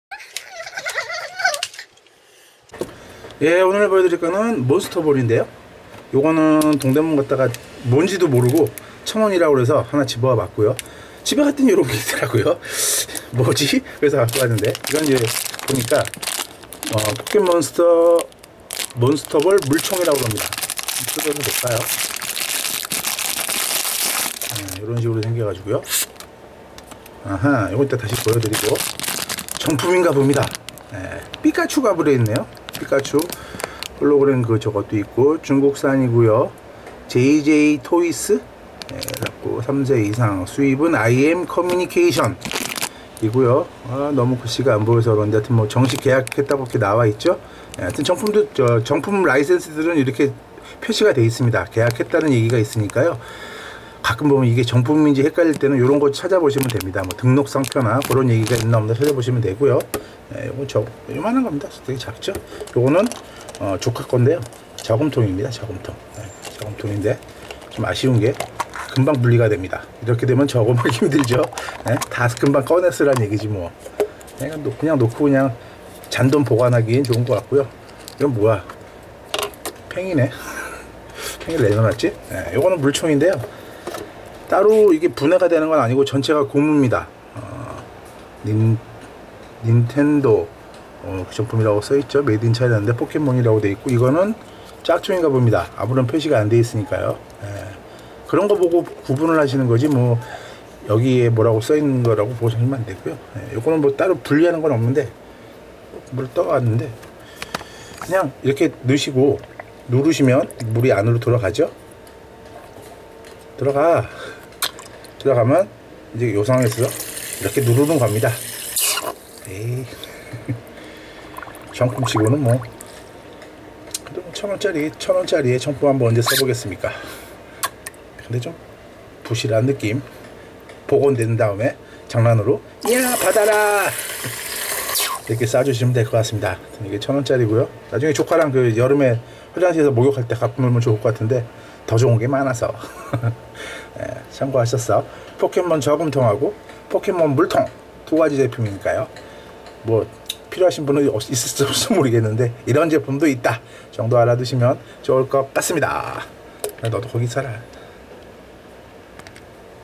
포켓 몬스터볼 물총과 저금통과 팽이 장난감 완구 제품 pocket monster ball Toy
동대문과 동네 문구점에서 구입한 제품인데, 포켓몬스터볼 처럼 생겨서 하나는 목욕탕이나 수영장 물가에서 사용할수 있는 미니 물총이 되고, 하나는 동그란 플라스틱속에 팽이가 두개 들어있고, 저금통으로도 사용할수 있는 제품입니다.
아주 마음에 들거나 괜찮다는 생각이 드는 정도는 아닌데, 이런 제품도 있다 정도 알아두시면 좋을듯하네요~

가격은 천원에서 2천원정도로 문방구, 문구점, 천원샵 등에서 판매를 하니 참고해 보시기 바라겠습니다~